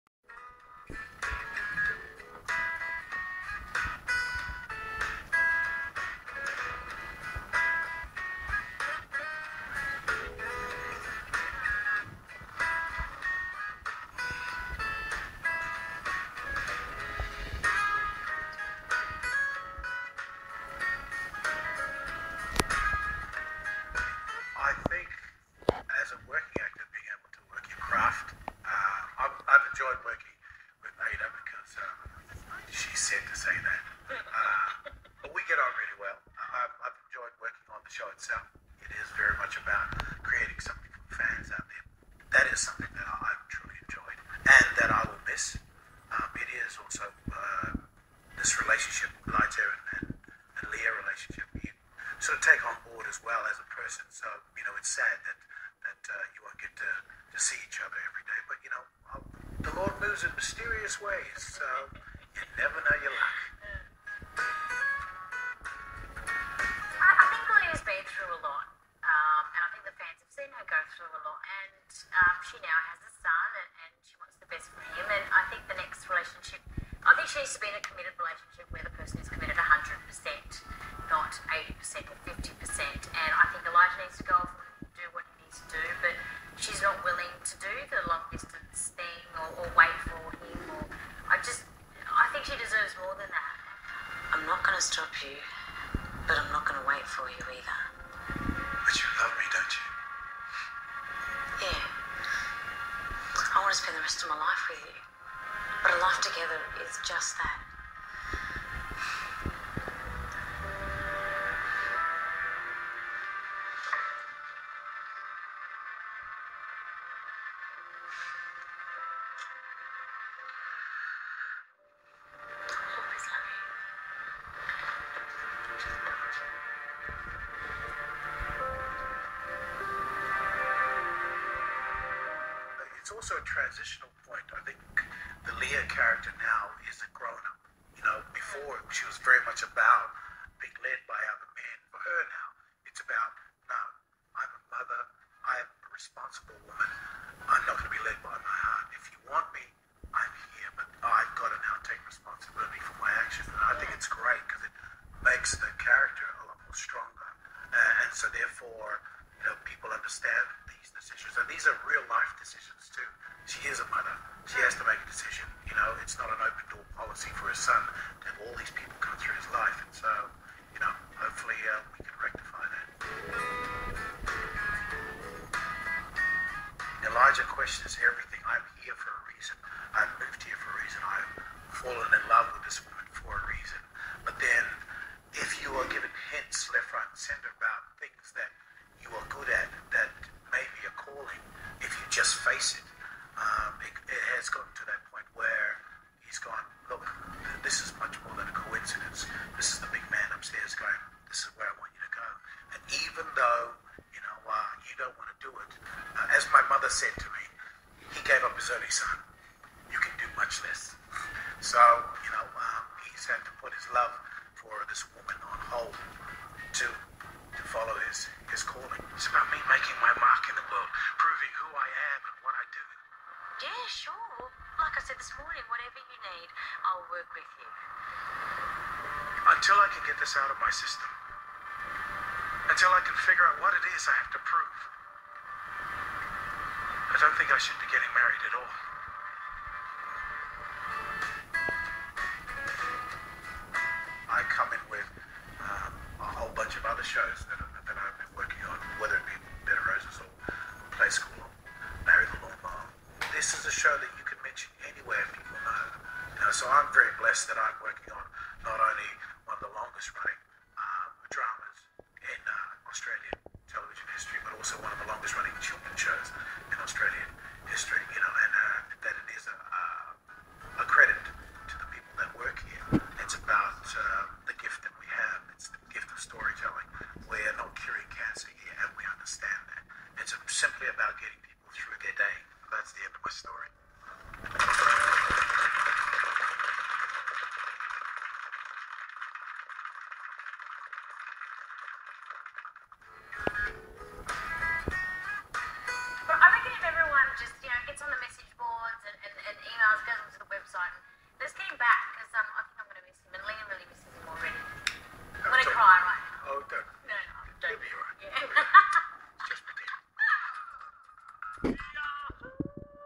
Home and away inerview with Elijah-Jay Laga' aia and Leah
interview of Elijah saying good bye to home and away, but most people say he maybe come back for short time, because he has lots other work in Tv so he is busy man.